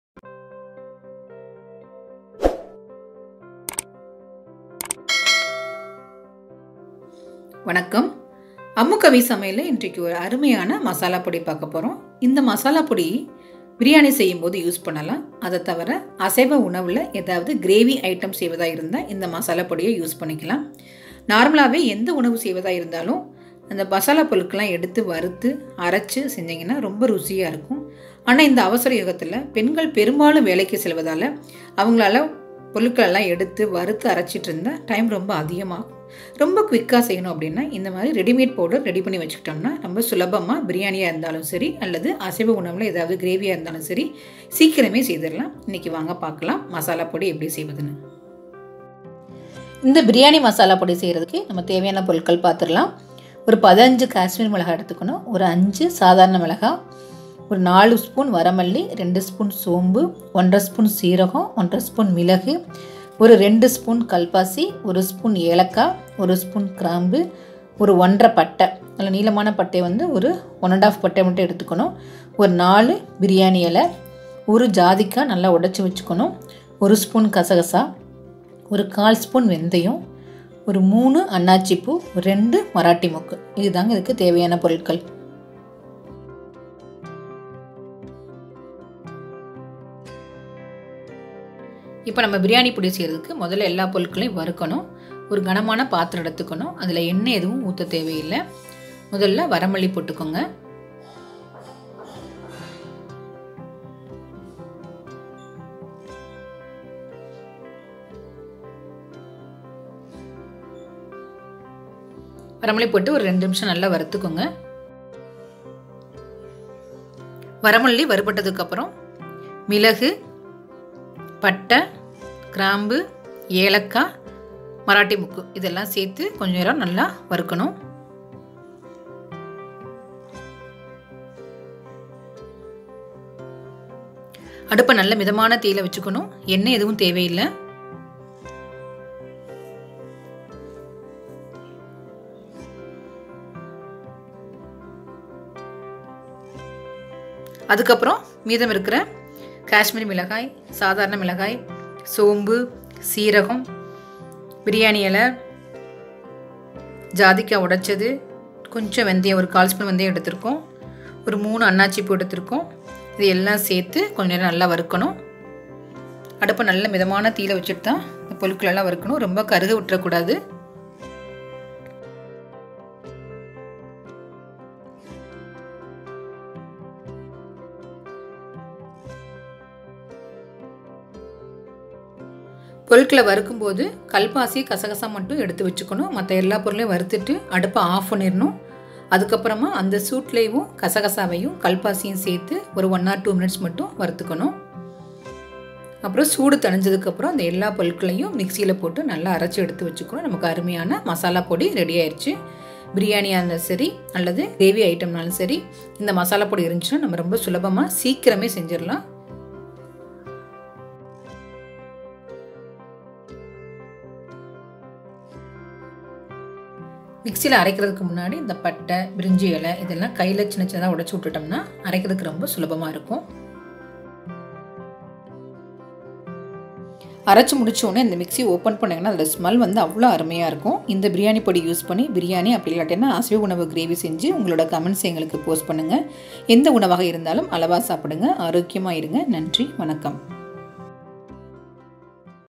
பிரியாணி சுவையாக இருக்க வேண்டுமா? How to make Biryani masala powder/Ammu Kavi Samayal/BiriyaniMasala
This homemade biriyani masala powder is flavorful and can be used to make any biriyani or non veg gravy items. While roasting the ingredients itself the smell was soo good. Biriyani masala powder should be stored in airtight container. Let's see in this video how to make biryani masala powder.